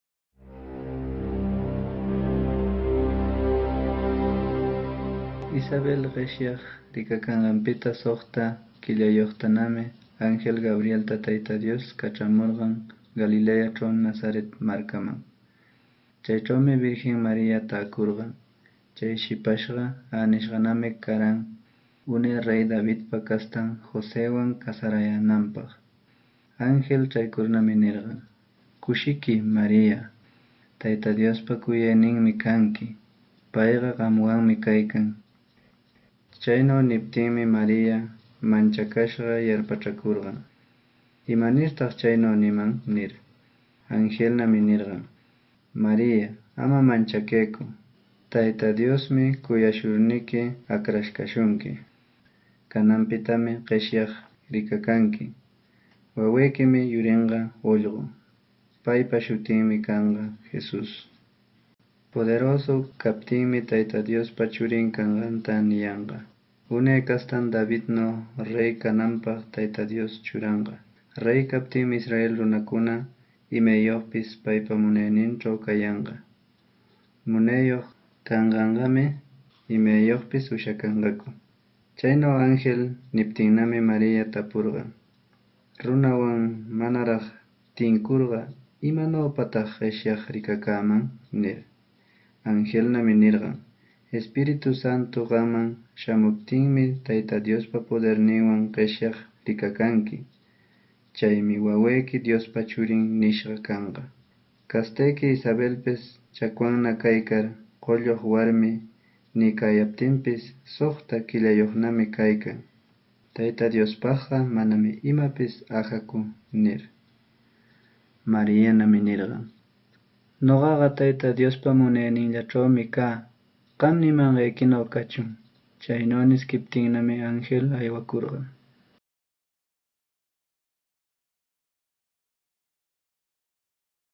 Quechua de Sihuas Ancash - Lucas 1.26-38 “Un ángel anuncia el nacimiento de Jesús” [qws]
Videos de las Escrituras en la lengua de Quechua de Sihuas Ancash de Szzz. [qws]
Visita ScriptureEarth para más materiales.